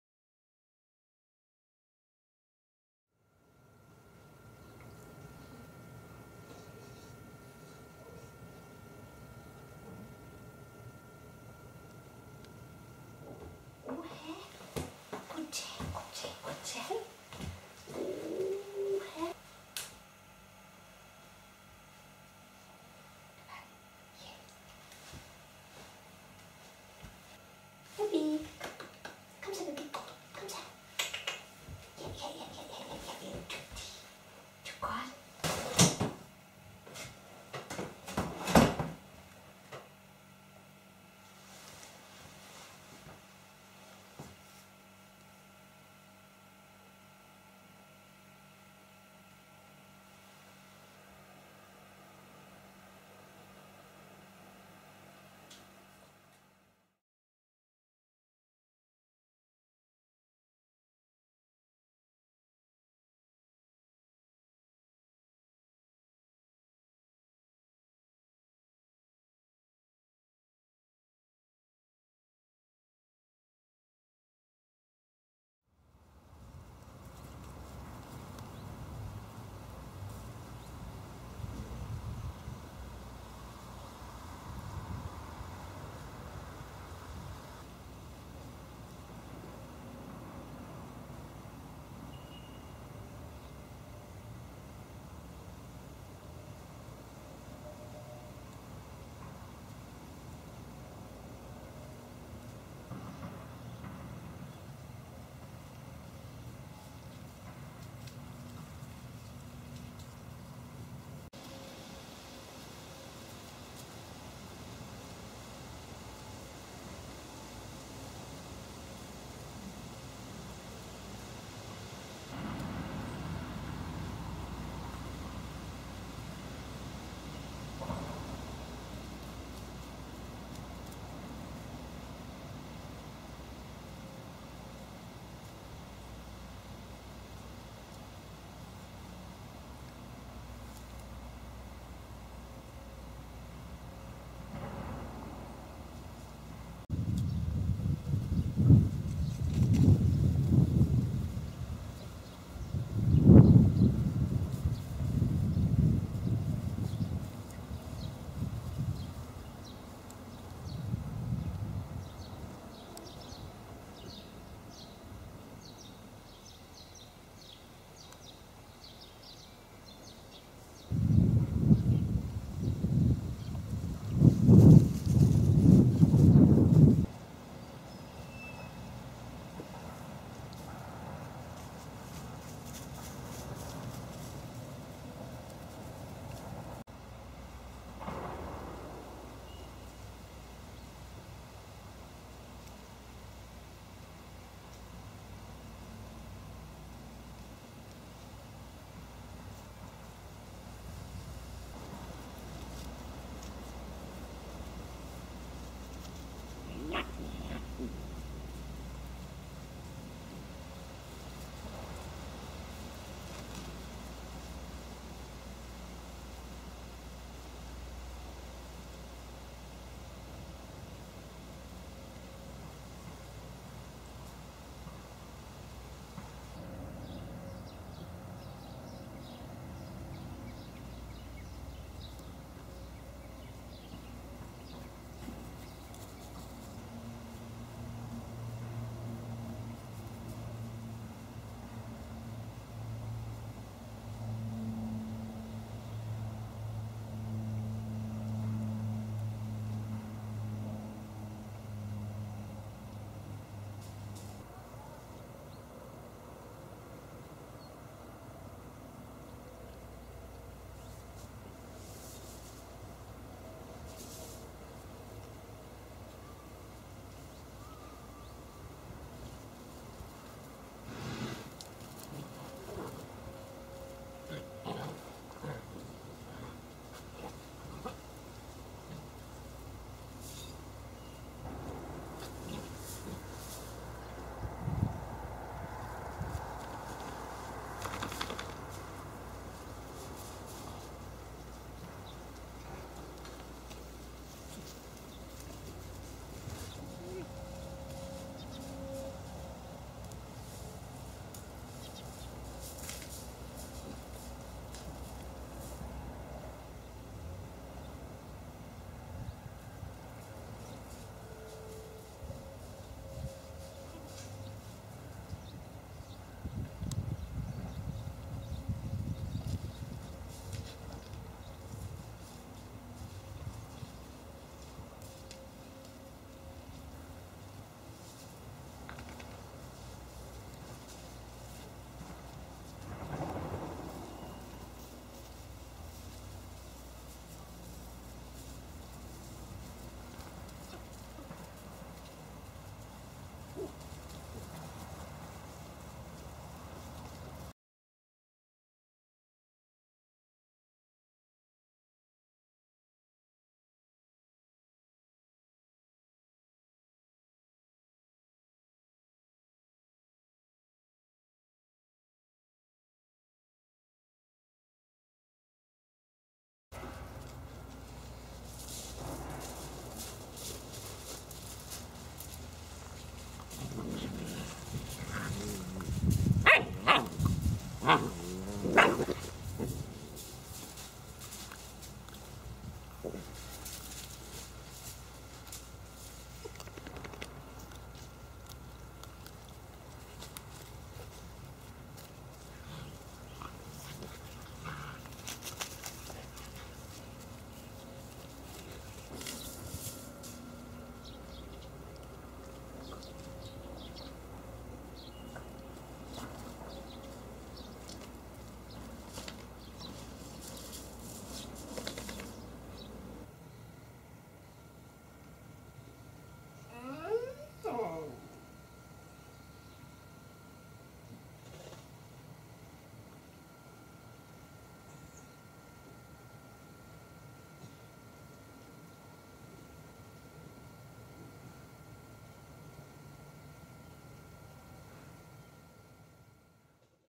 Ulvsholms M-kuld 8 uger
"Aldrig havde vi troet, vi skulle komme ud for så meget på 1 uge!"
Ulvsholms M-kuld født 2. august 2013 - 8 uger gamle.